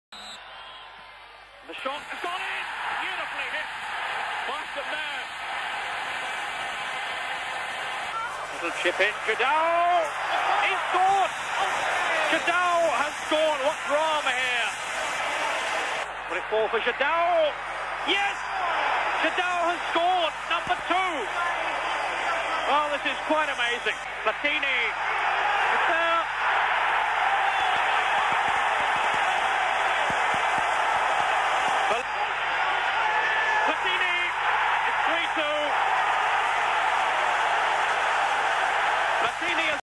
Francia 3 2 Portugal - Eurocopa Francia 1984
Apasionante semifinal. Un partidazo a recordar, con la selección francesa de Platini en su cénit futbolístico, y con una Portugal rayando la excelencia. Merecieron pasar los dos.